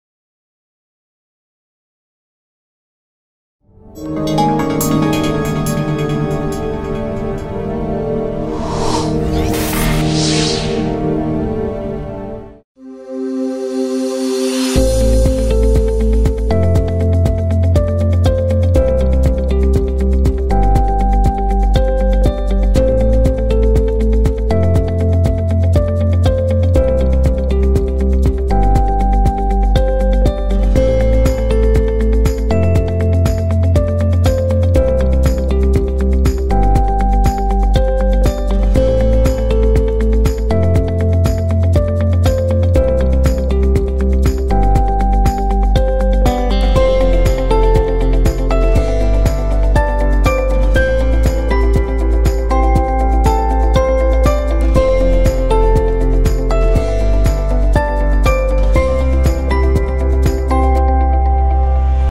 AAVISHKAR ADVANCE EYE HOSPITAL & CORNEA CENTRE | Advertisement shoot | ruhgraphy
AAVISHKAR Hospital is a high class eye medical facility.